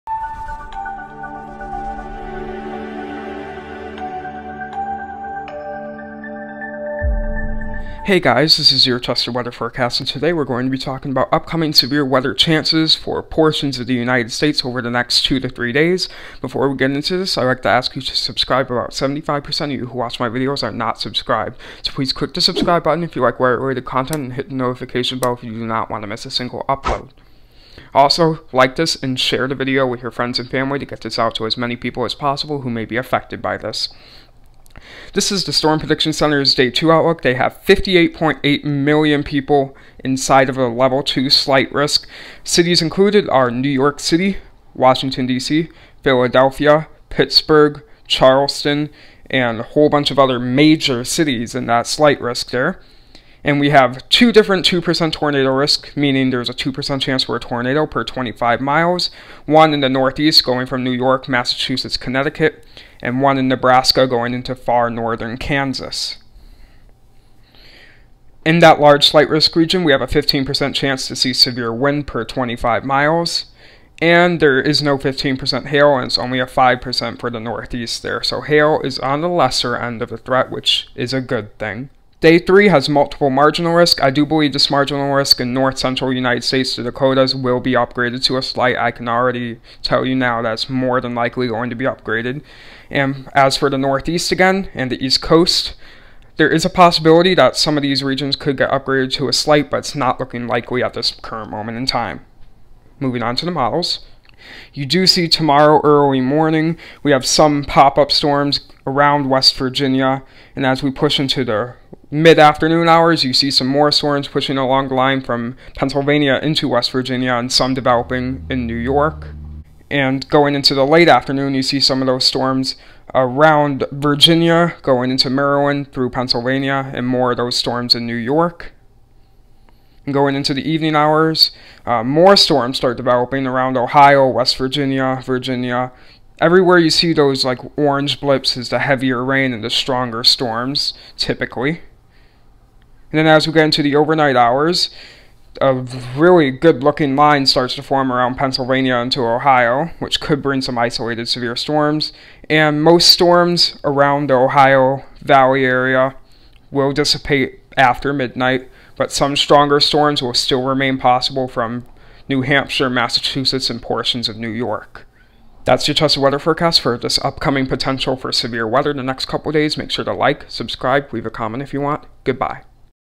Upcoming Severe Chances (7/22-7/23)
Chances for severe weather will happen in portions of the North-East on Wednesday and portions of the Dakotas on Thursday. Some of these storms could produce tornadoes, otherwise, damaging winds will be the main hazard.